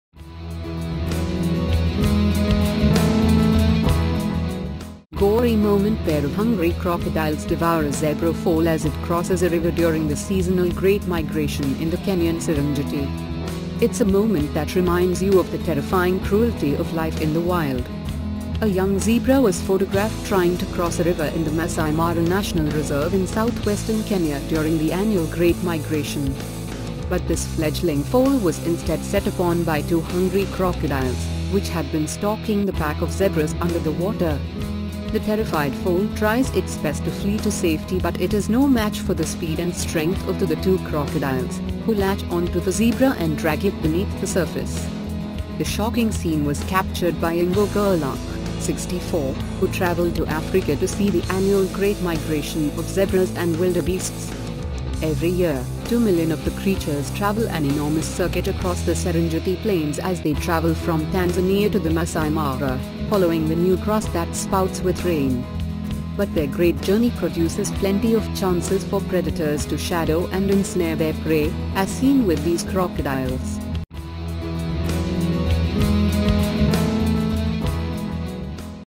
Gory moment pair of hungry crocodiles devour a zebra foal as it crosses a river during the seasonal
Gory moment pair of hungry crocodiles devour a zebra foal as it crosses a river during the seasonal ‘Great Migration’ in the Kenyan Serengeti

It's a moment that reminds you of the terrifying cruelty of life in the wild.
A young zebra was photographed trying to cross a river in the Maasai Mara National Reserve in south western Kenya during the annual 'Great Migration'.
But this fledgling foal was instead set upon by two hungry crocodiles, which had been stalking the pack of zebras under the water.
The terrified foal tries its best to flee to safety but it is no match for the speed and strength of the the two crocodiles, who latch on to the zebra and drag it beneath the surface.
The shocking scene was captured by Ingo Gerlach, 64, who traveled to Africa to see the annual 'Great Migration of zebras and wildebeests.
Every year, two million of the creatures travel an enormous circuit across the Serengeti Plains as they travel from Tanzania to the Maasai Mara, following the new grass that spouts with rain.
But their great journey produces plenty of chances for predators to shadow and ensnare their prey, as seen with these crocodiles.